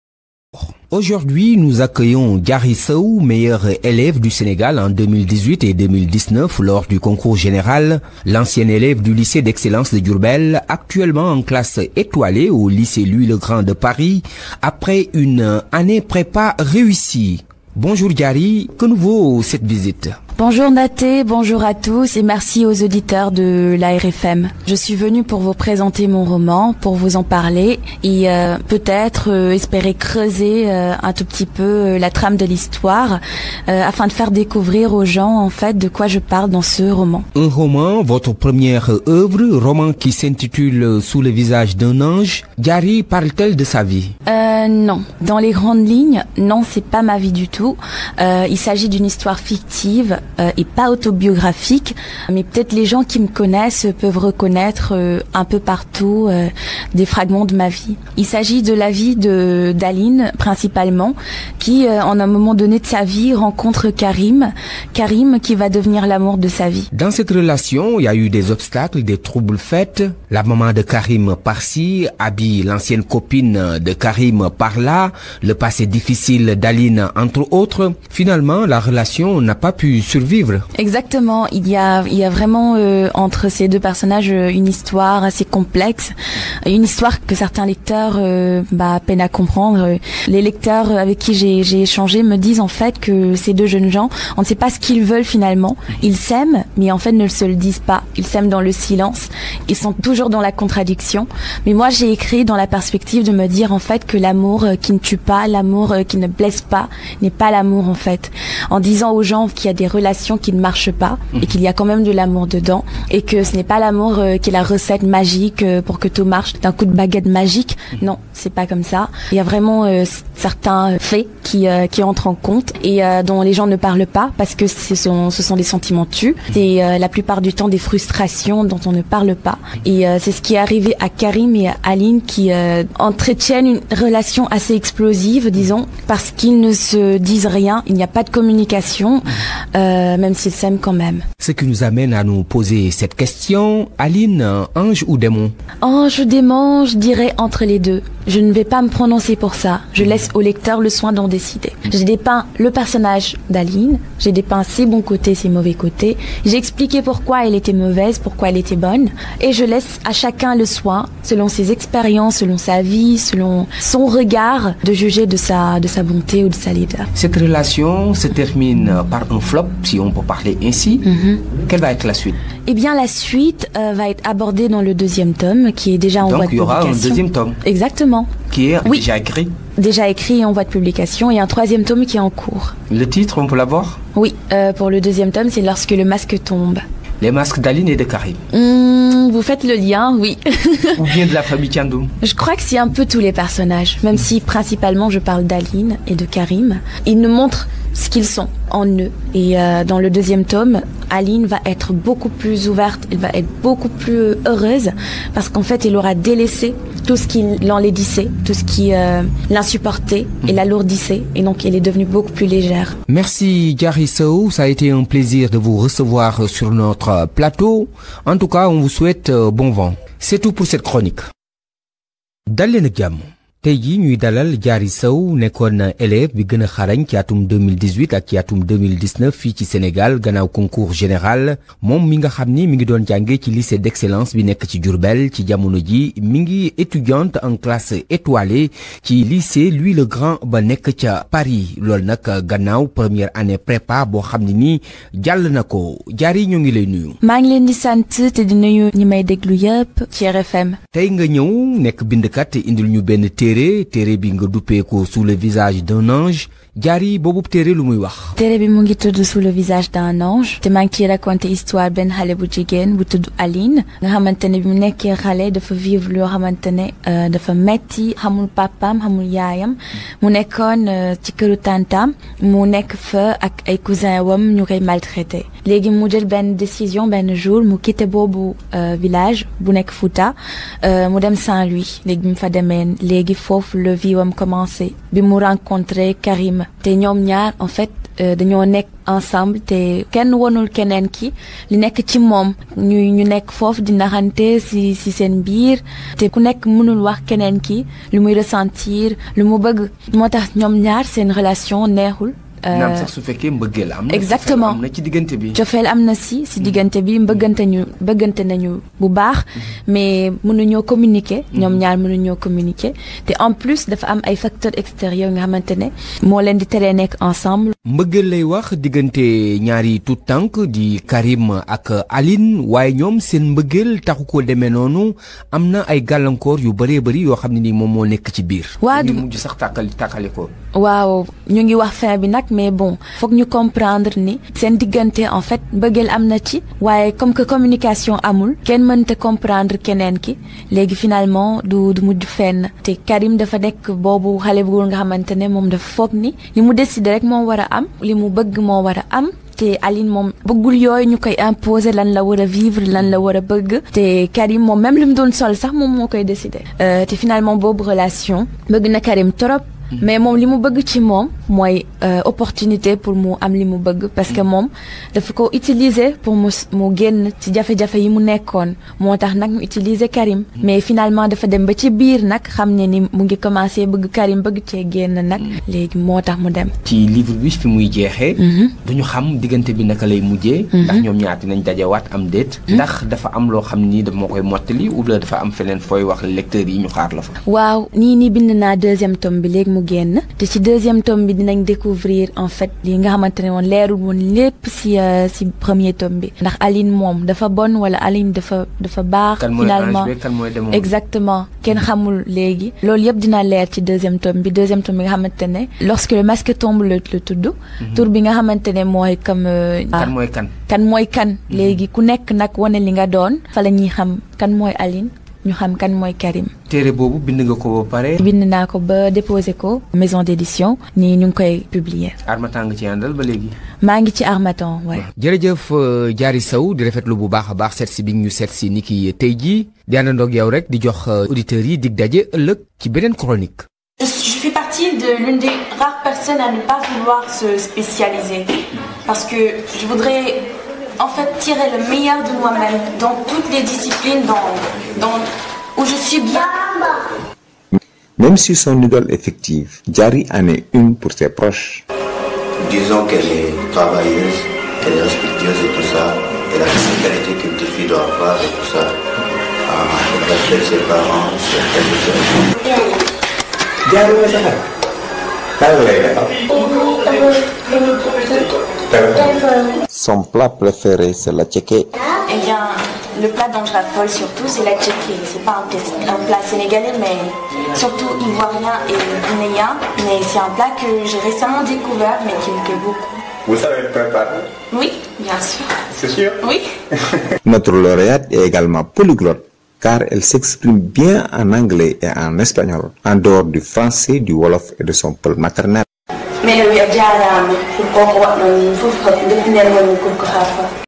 Passage Rfm Diary Sow Wolof ET Français "Lorsque les masques tombent"
Diary sow sous le visage d'un ange
"Lorsque les masques tombent" : Diary s'apprêtait à sortir son 2e roman